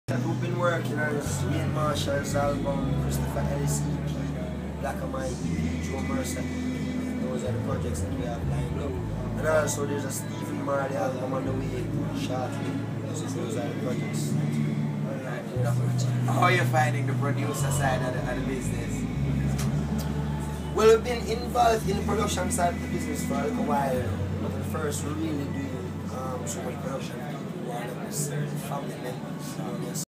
Jr Gong interview
Snippet of an interview I did with Damian Jr Gong Marley after his Reggae Sumfest 2013 interview